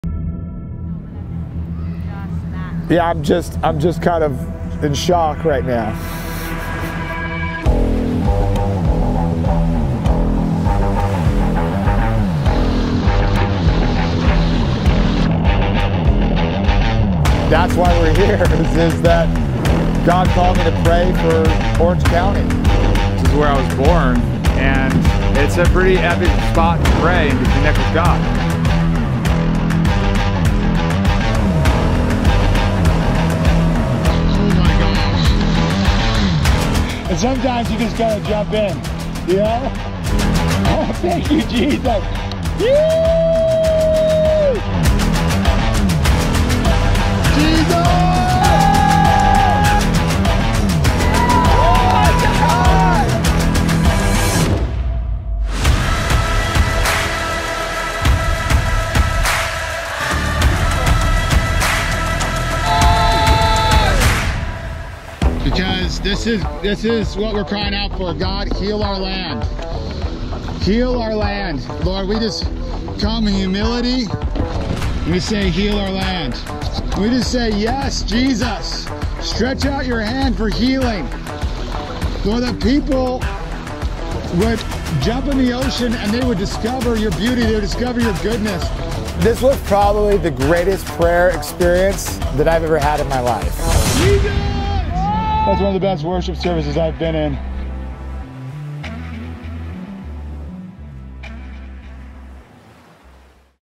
Catch The Wave PRAYER DOCUMENTARY TRAILER
It's been almost a year in the making. Over 150 miles paddled and over 5,000 hours spent on the water praying for the state of California, America, and the world since February of 2020 while smack dab in the middle of lockdown. This was all birthed from a dream where The Lord called us to intercede. This is a movement that sees dreams realized. Join the new prayer movement. Heal our land. Say yes to the new wineskin of intercessors. Here we are Lord. Full episodes coming soon.